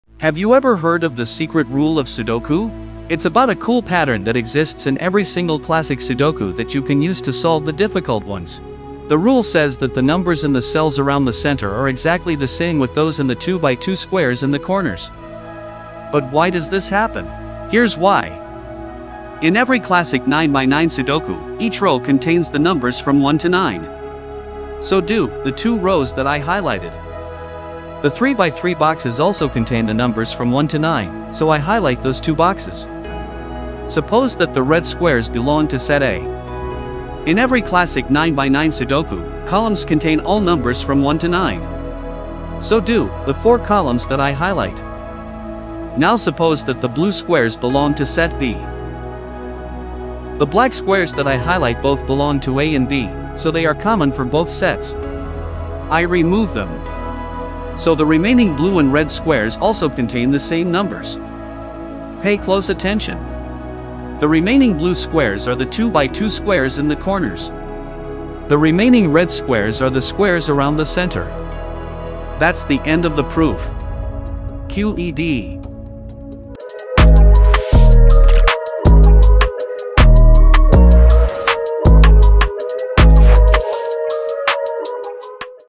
The SECRET Rule of Sudoku that NOBODY KNOWS!!!
Apply this rule to high difficulty sudokus in the middle of the solution when you get stuck and in some cases you may even find the solution! Remember, even though the rule works for every sudoku, it will not always be the ideal tactic! if you wanna be able to find the best tactics in sudokus, subscribe my channel and watch my sudoku videos.